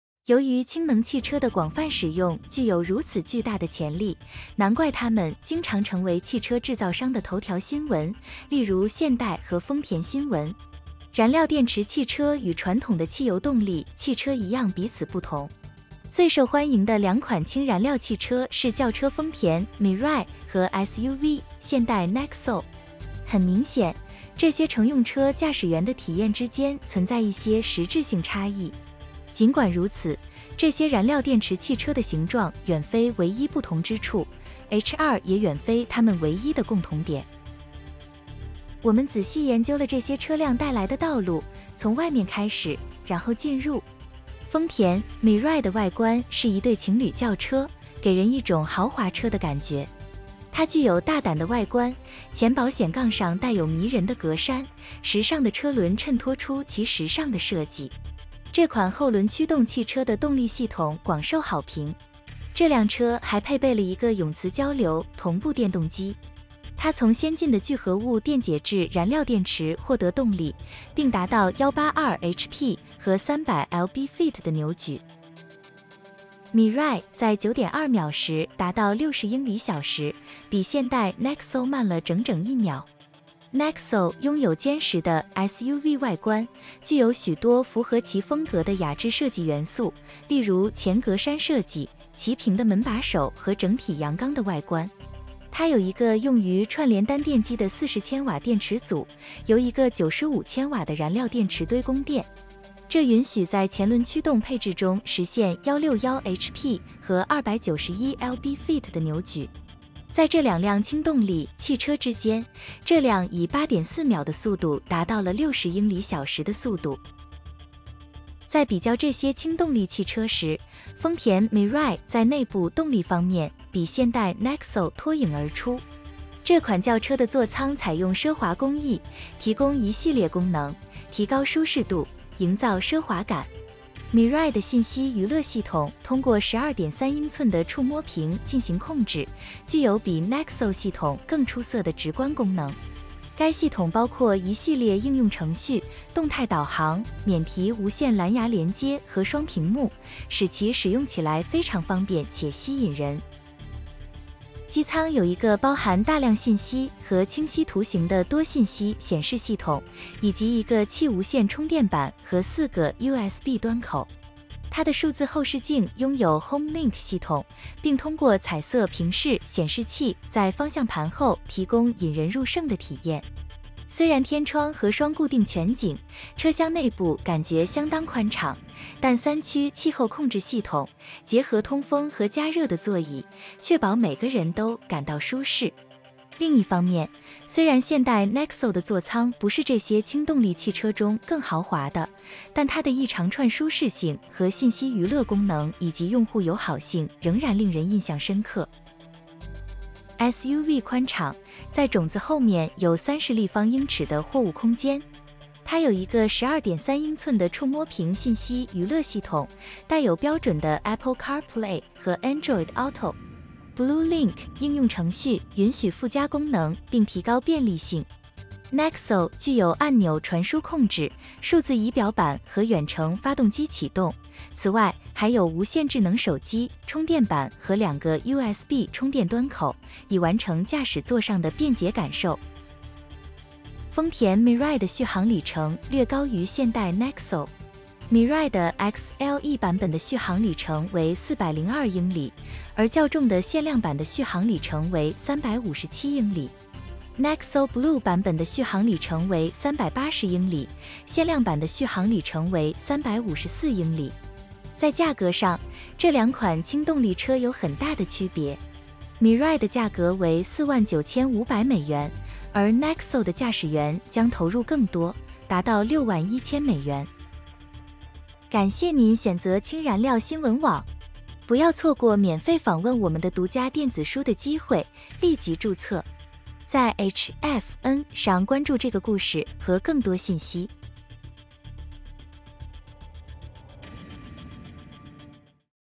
[Chinese] Toyota Mirai vs Hyundai Nexo: How these hydrogen cars compare
These top fuel cell vehicles get lots of time in the spotlight, but how are they similar and different?